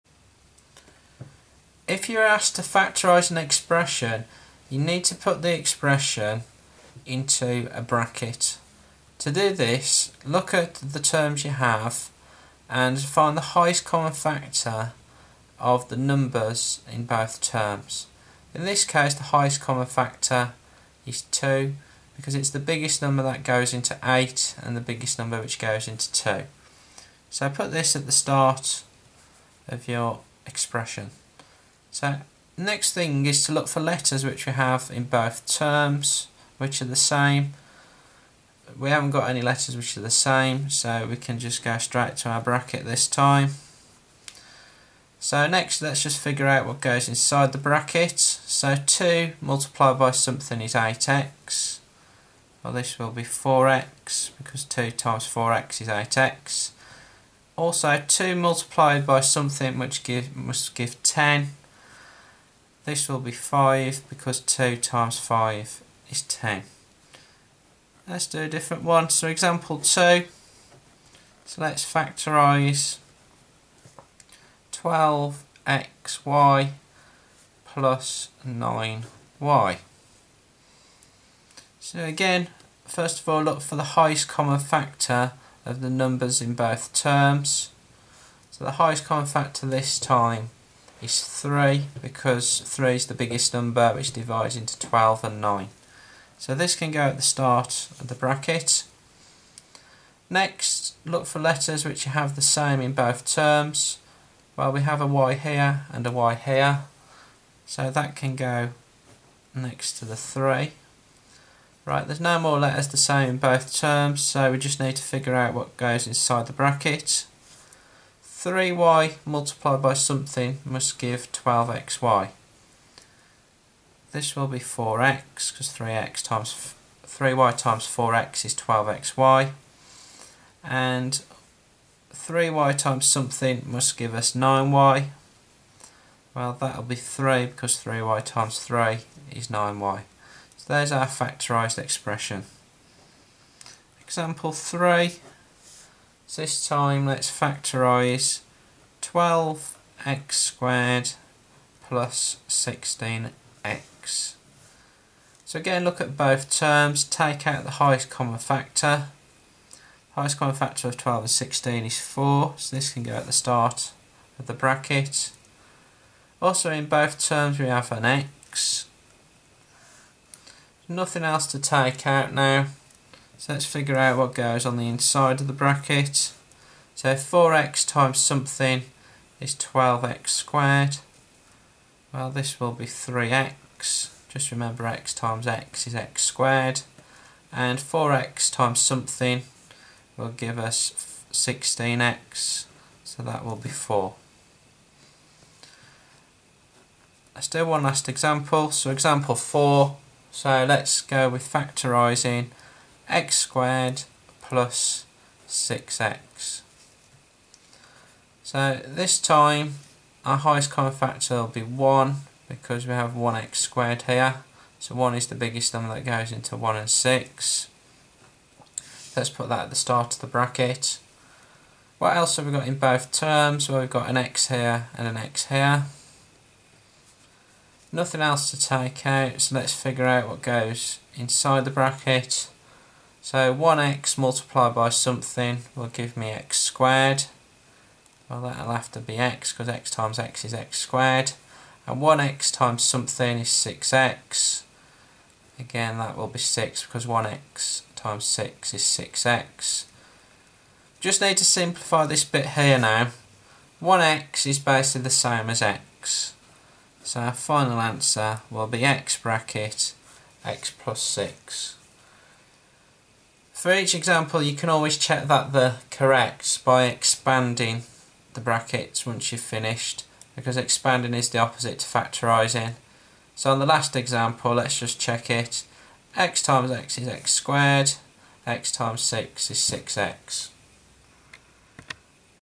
Factorising single bracket expressions. A quick guide with 4 examples.
Here you will learn how to carry out factorisation of expressions which go into single brackets. You can do this by looking for the highest common factor of both numbers in the expression. Put this at the start of the bracket. Also look for any letters the same in both terms and also put these at the start of the bracket. Finally, work out the inside terms of the bracket by thinking about what will need to multiply the term at the start of the bracket to give the original expression.

Examples include factorisng 8x + 10 which is 2(4x +5) and factorising 12xy + 9y